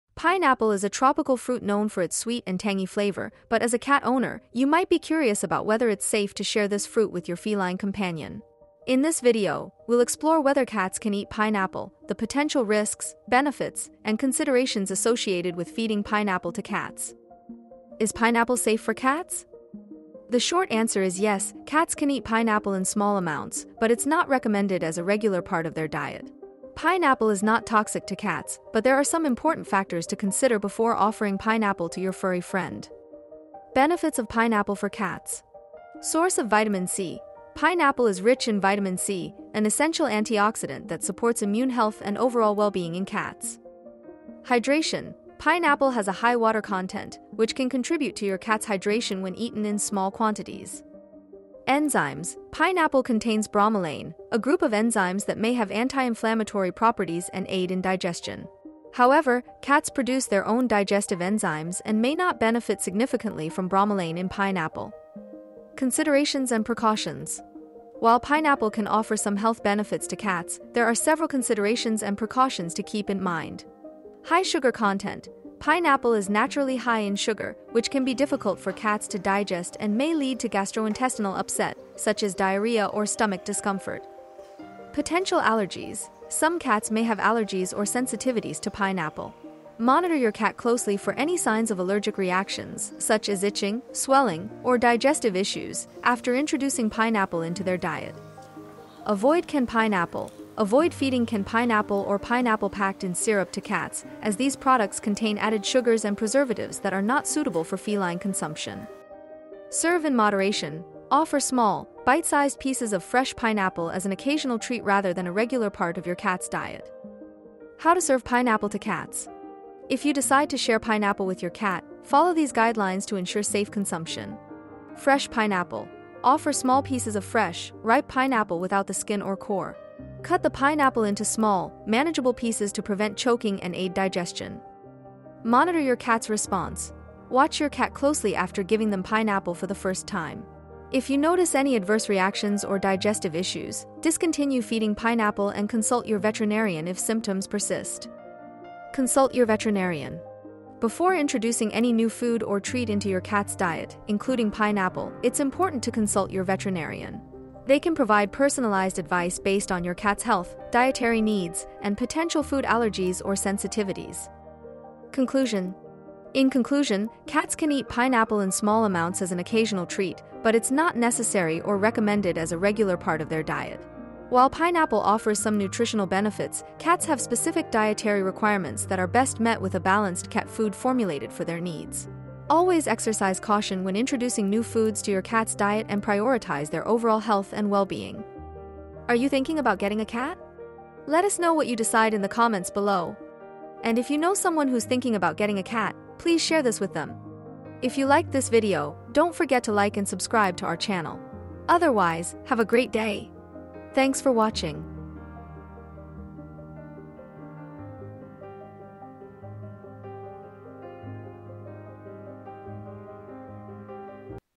Can Cats Eat Pineapple? 🍍🐱
Discover whether pineapple is a suitable treat for your feline companion in this informative video! If you're a cat owner curious about incorporating pineapple into your cat’s diet or concerned about its potential effects, this video provides a thorough exploration of the topic. Learn about the unique aspects of cats' digestive systems, their specific dietary needs, and the nutritional benefits and risks associated with feeding pineapple. Gain expert insights into the pros and cons of offering fruits like pineapple to your cat, along with best practices to ensure their health and safety. Whether you're a cat enthusiast or simply looking for reliable information on feline nutrition, this video offers valuable guidance on the relationship between cats and pineapple! 🍍🐱